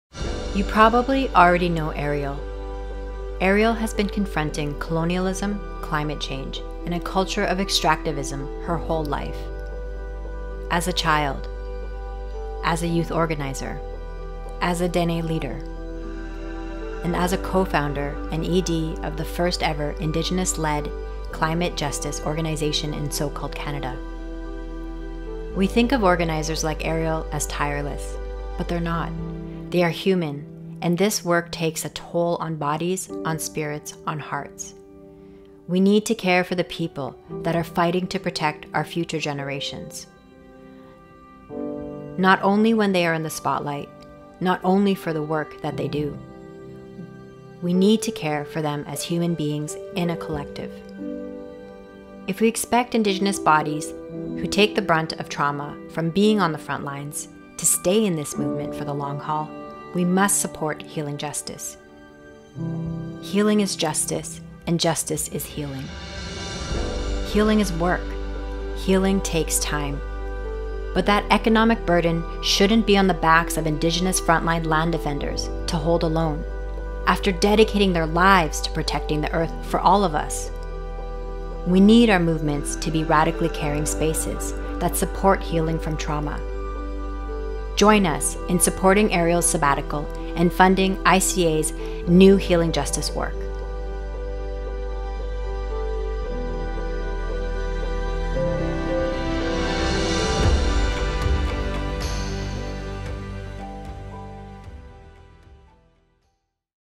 Healing is Justice: Eriel’s Sabbatical
Our Executive Director, Eriel Tchekwie Deranger, has recently gone on sabbatical. This video speaks to the need for organizers to take time for healing, and the important of healing justice within climate work. If you're able to, please donate to Eriel's sabbatical and our emerging healing justice work.

Voiceover by Melina Laboucan-Massimo.